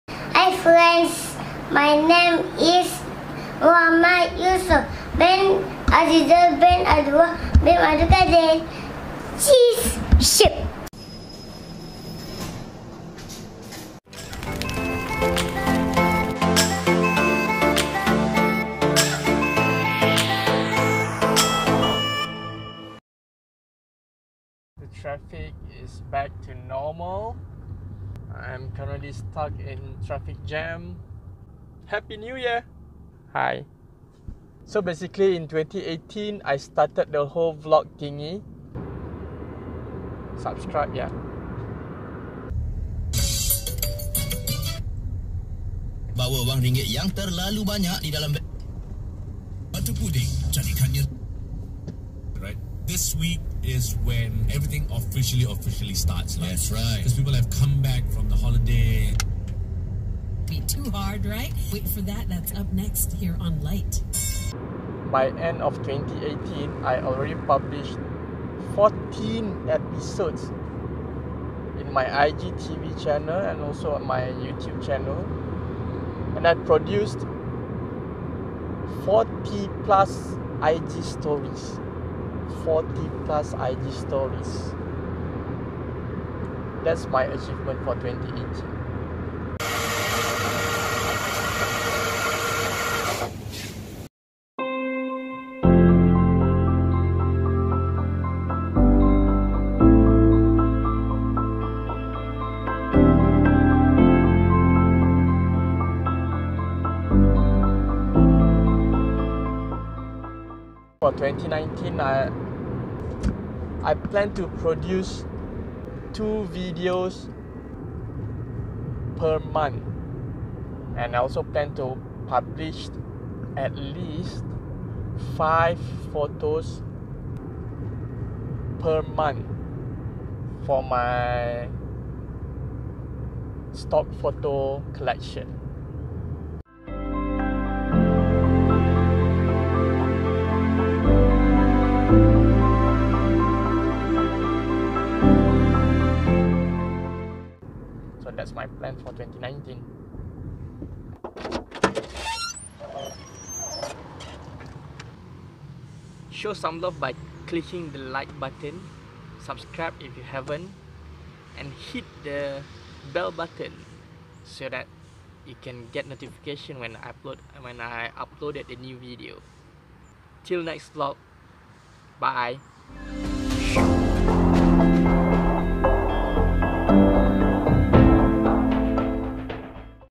vlog #1 - Bye 2018, Welcome 2019
My first vlog for 2019. It's about what I have achieved in 2018 and my plan for 2019.

My Primary Camera
My Secondary Camera
The Mic That I Use
The Memory Card
The Editing Software: PowerDirector Mobile